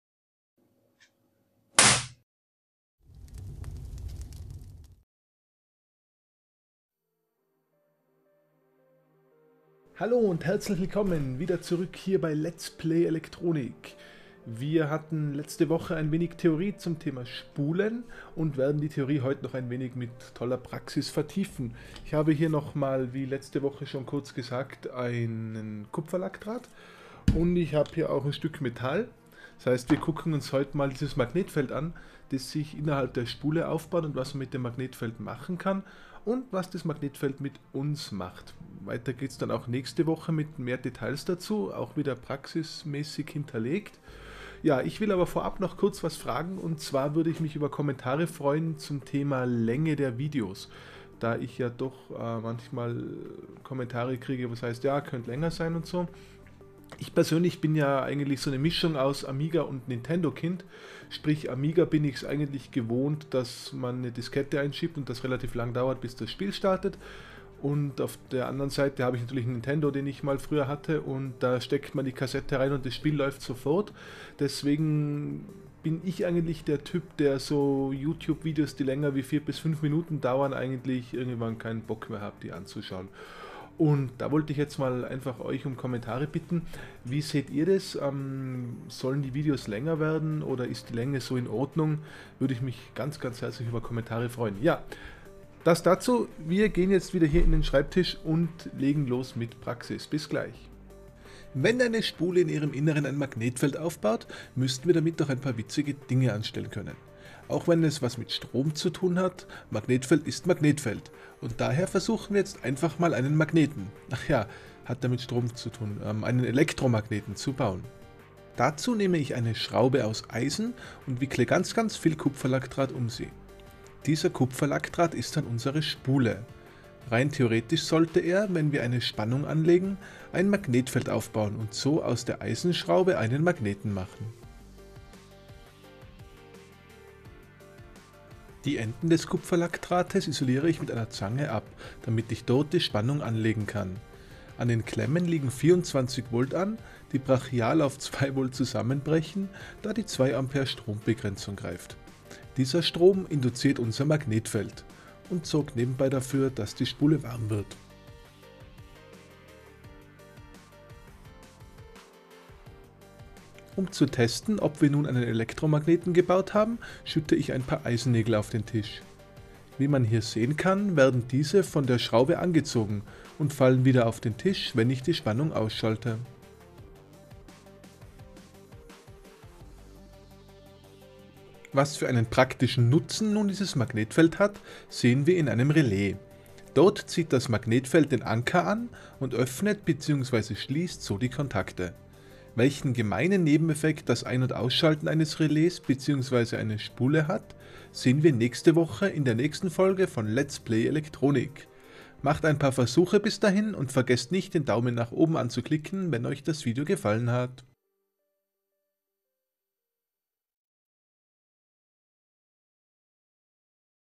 🏮 Elektromagnet selber bauen | Let's play electronic #014
In diesem Video nutzen wir das Magnetfeld, das eine Spule erzeugt und versuchen daraus einen Elektromagneten zu bauen.

📆 Immer am Ersten des Monats:
ein neues Video zum Thema Elektronik, Mikrocontroller, ...